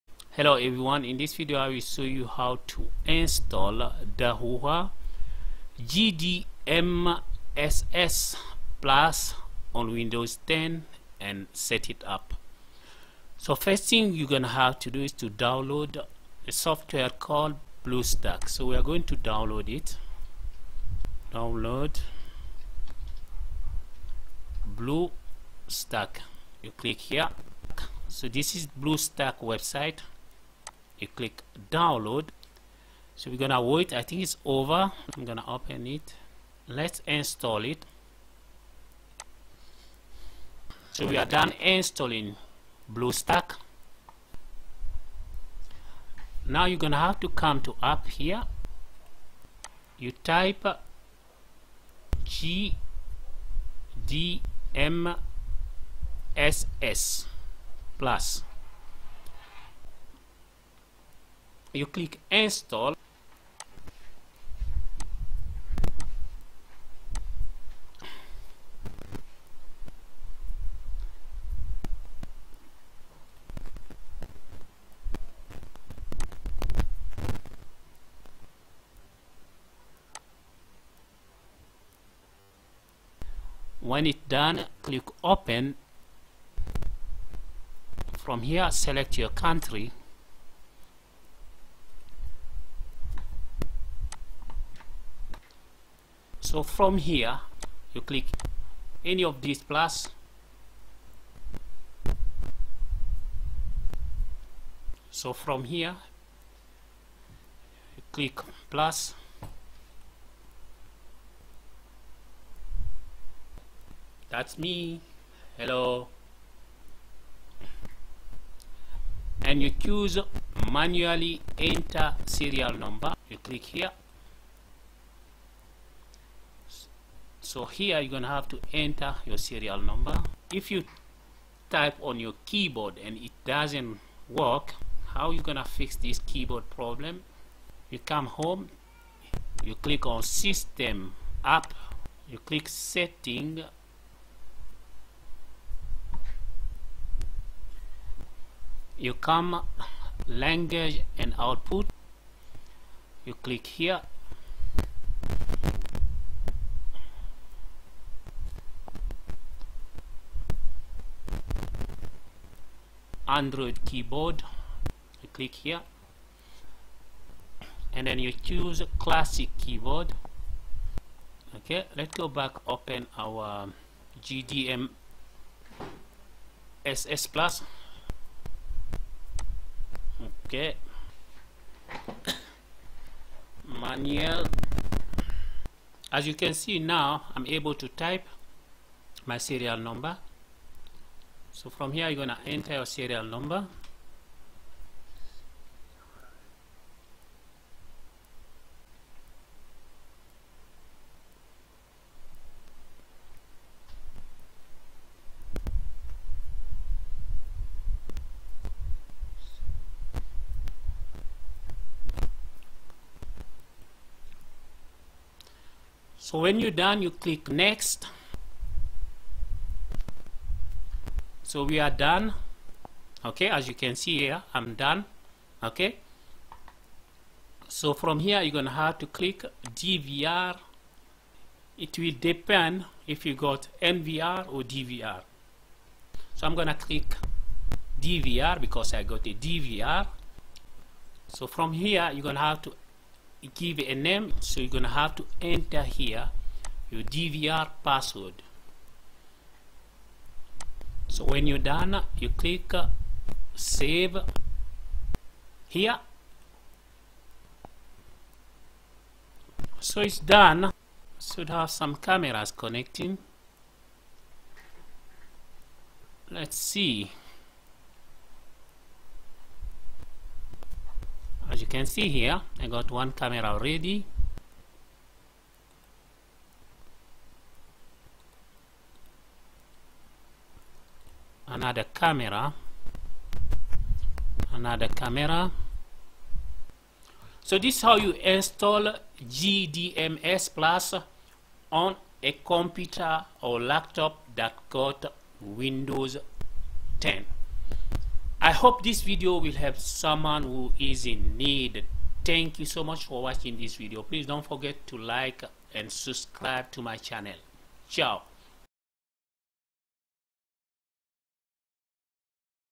gdmss plus for pc: How to install gdmss plus on computer
gDMSS Plus for pc: In this video, you will learn how to download, install, and set up the gDMSS plus app on a computer or laptop.